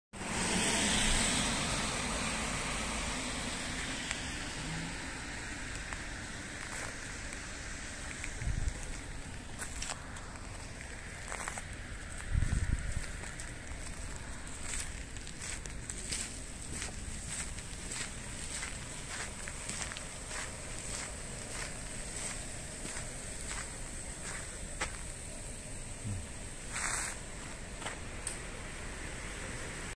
国府宮神社の夜桜照明の様子
国府宮神社参道の夜桜照明の様子です

令和4年4月3日は寒かった

ときどき小雨がパラパラっと降ってたので

人も少なく

おかげで雰囲気があってよかったです。

名古屋市の中古マンション専門のホームページ